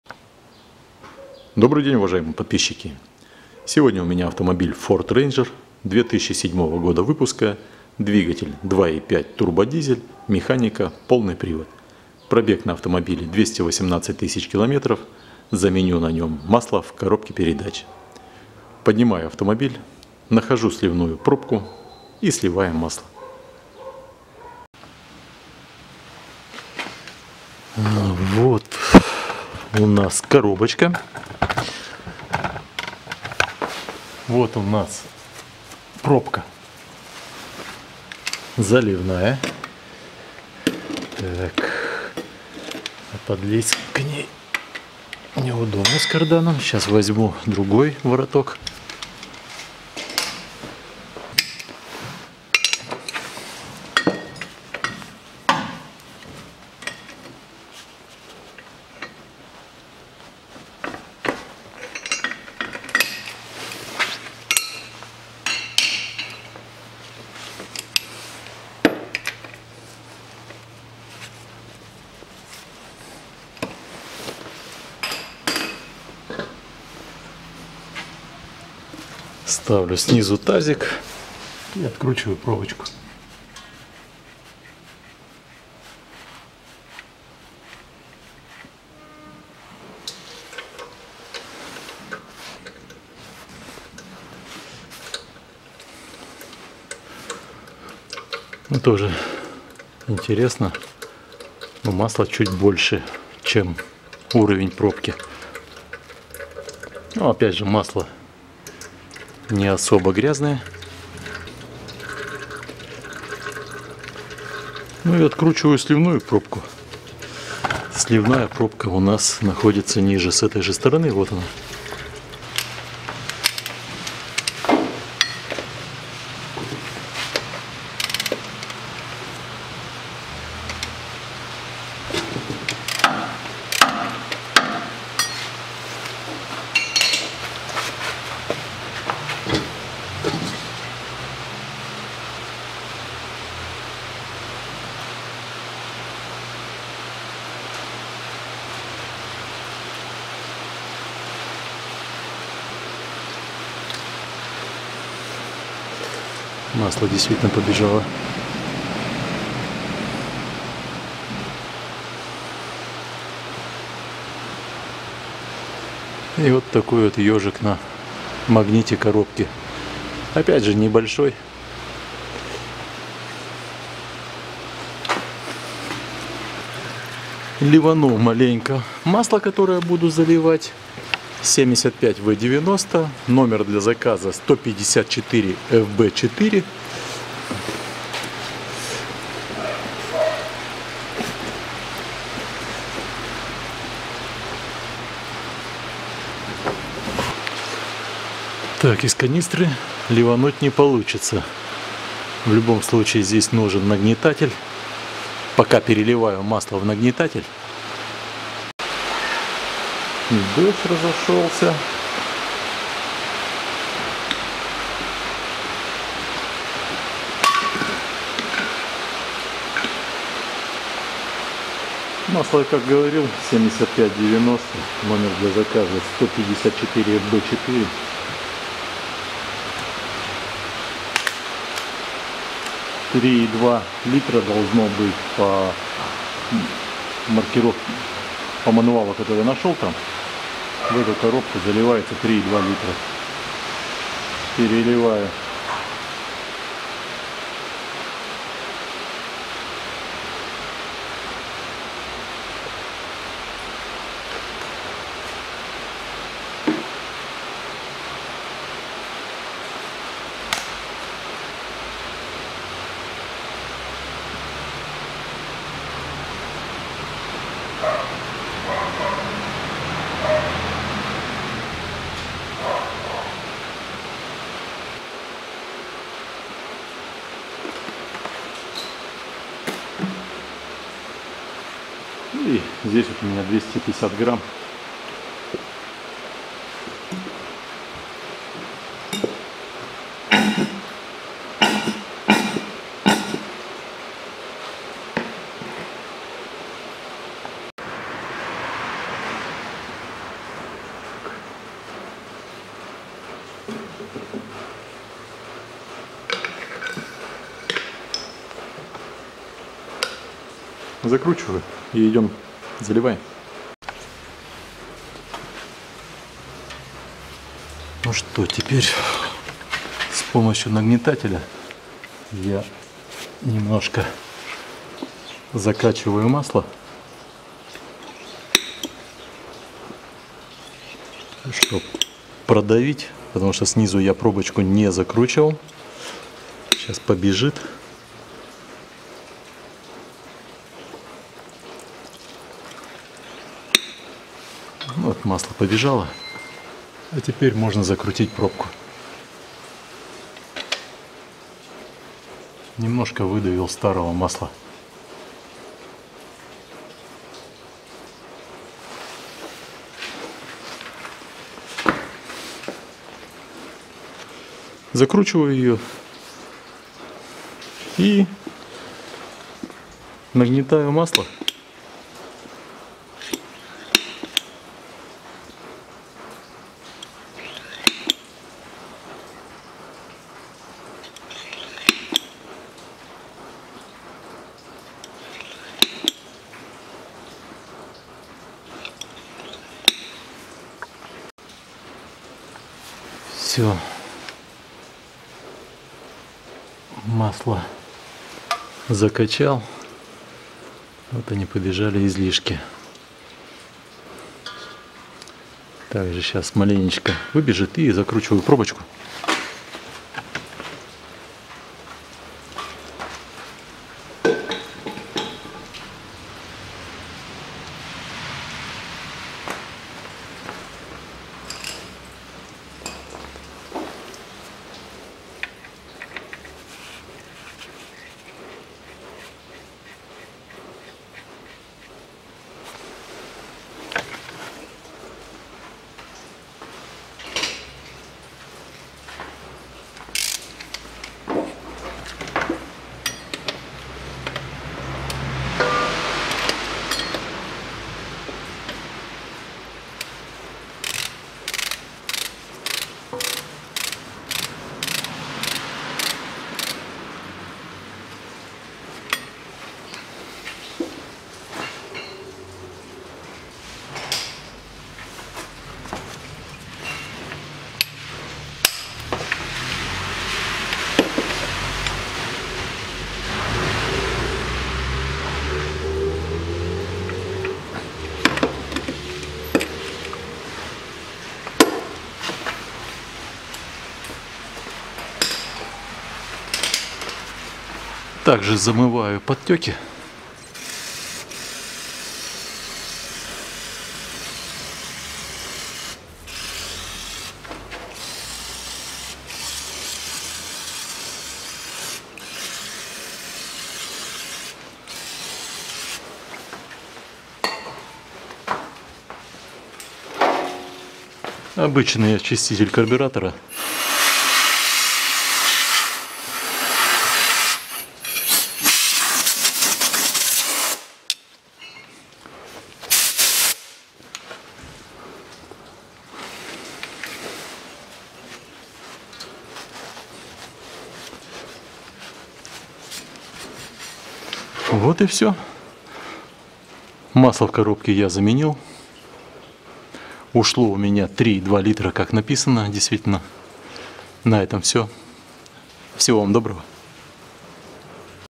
Замена масла в коробке передач на Ford Ranger 2,5 TURBO Форд Рейнджер 2007 года
Запчасти для своих нужд заказываю в Краснодаре в магазине TWIN-CAM по т. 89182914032

масло  154FB4  CASTROL  75w90    3,2 литра